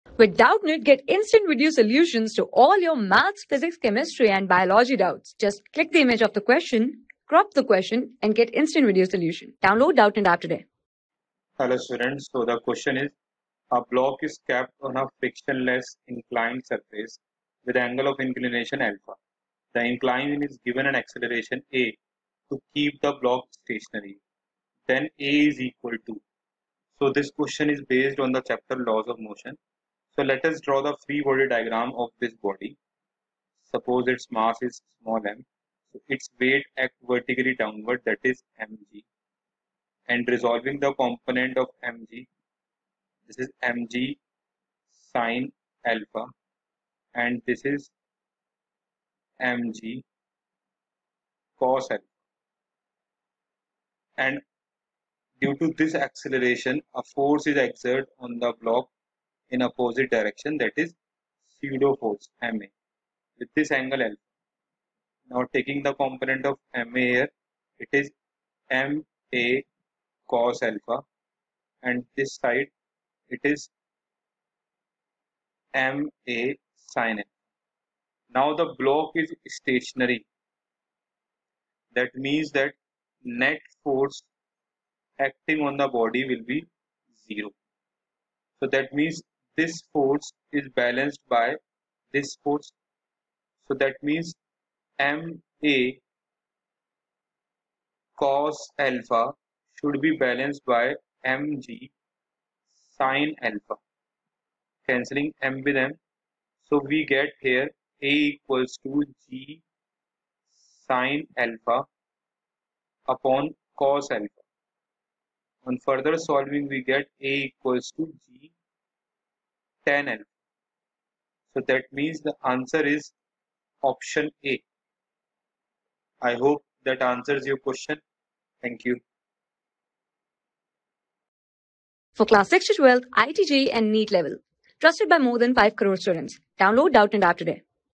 A block is kept on a frictionless inclined surface with angle of inclination alpha. The incline ...
A block is kept on a frictionless inclined surface with angle of inclination alpha. The incline is given an acceleration \'a\' to keep the block stationary. Then a is equal to
Class: 12
Subject: PHYSICS
Chapter: DYNAMICS OF A PARTICLE
Board:IIT JEE

👉 Have Any Query? Ask Us.